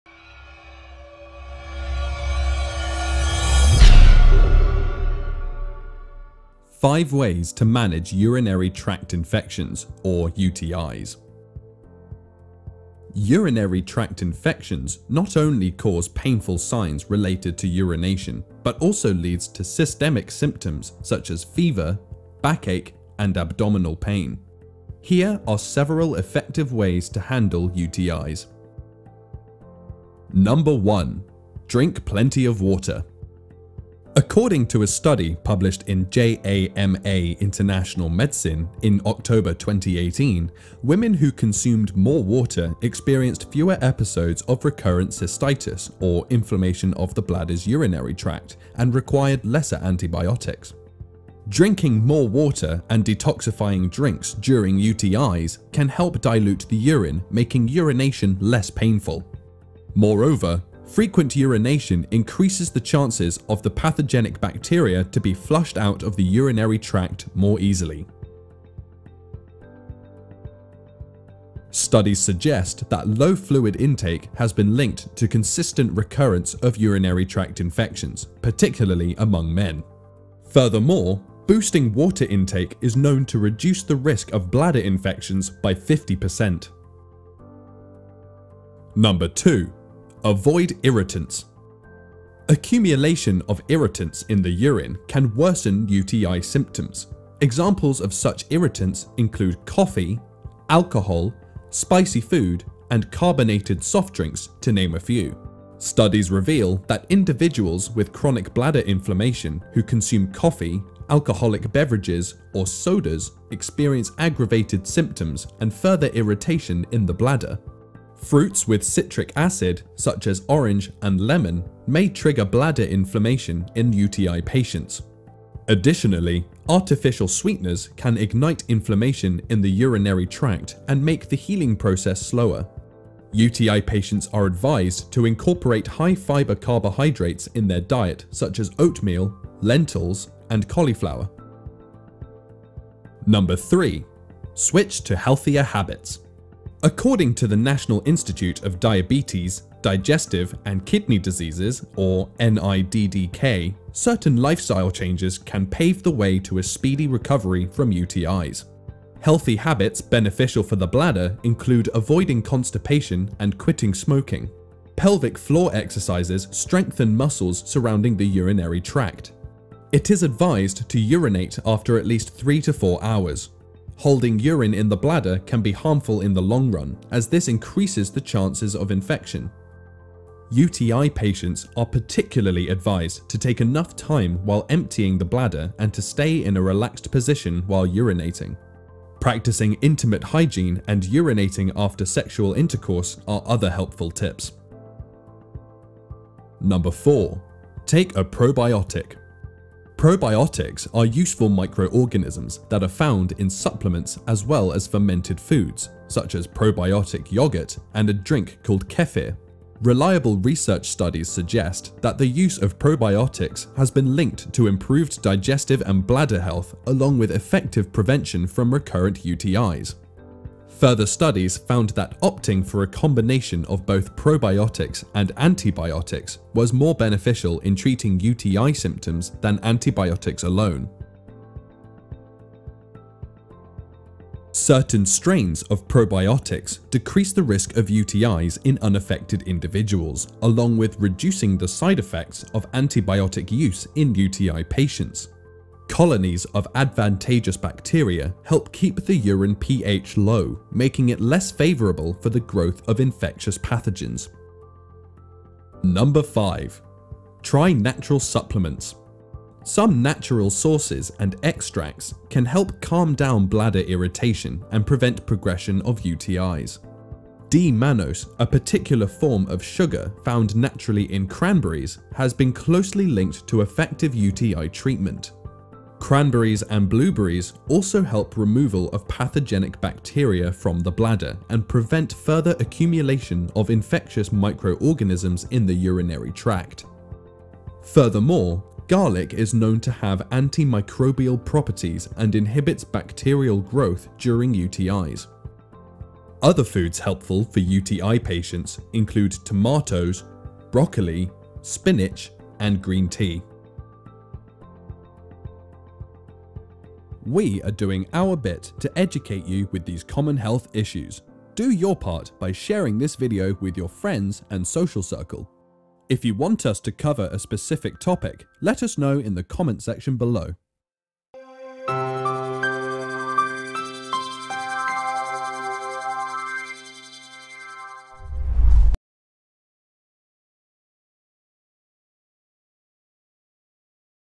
5 ways to manage Urinary Tract Infections
5 ways to manage Urinary Tract Infections

Urinary tract infection not only causes painful signs related to urination but also leads to systemic symptoms such as fever, back ache and abdominal pain. Here are several effective ways to handle UTI:

1. Drink plenty of water.
2. Avoid irritants.
3. Switch to healthier habits.
4. Take a probiotic.
5. Try natural supplements.

Drinking more water and detoxifying drinks during UTI can help dilute the urine, making urination less painful. Moreover, frequent urination increases the chances of the pathogenic bacteria to be flushed out of the urinary tract more easily. Accumulation of irritants in the urine can worsen UTI symptoms. Examples of such irritants include coffee, alcohol, spicy food and carbonated soft drinks to name a few. Healthy habits beneficial for the bladder include avoiding constipation and quitting smoking. Probiotics are useful microorganisms that are found in supplements as well as fermented foods such as probiotic yogurt and a drink called kefir. Reliable researches suggest that the use of probiotics has been linked to improved digestive and bladder health along with effective prevention from recurrent UTI Cranberries and blueberries also help removal of pathogenic bacteria from the bladder and prevent further accumulation of infectious microorganisms in the urinary tract. Furthermore, garlic is known to have anti-microbial properties and inhibits bacterial growth during UTI.